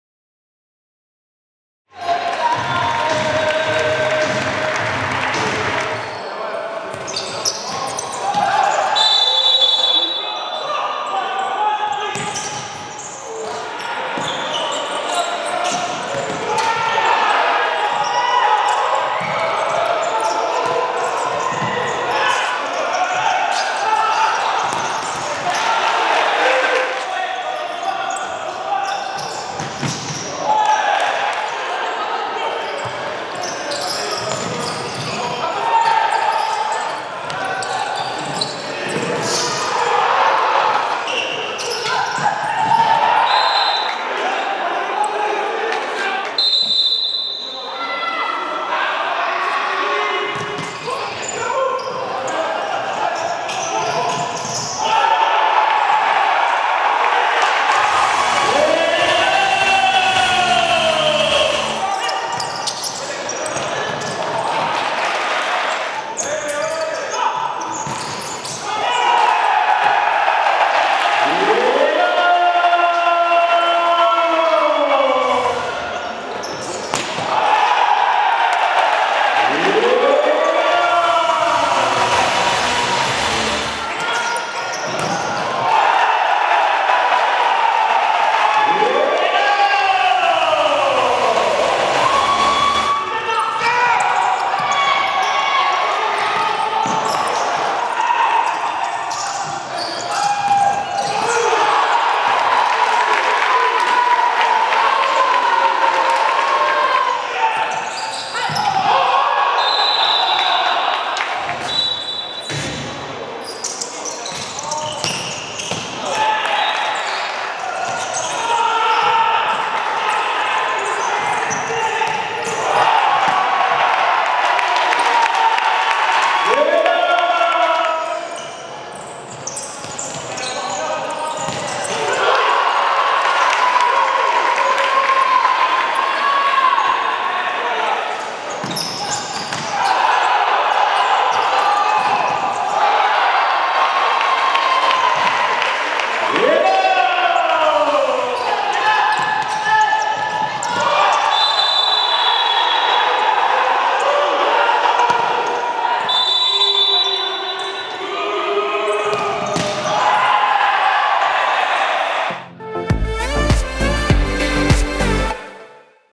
J1 : Sporting Paris - Garges Ddjibson (6-2)
Découvrez le résumé de la rencontre entre le SPorting Club Paris et Garges Djibson comptant pour la 1ère journée du Championnat de France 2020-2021.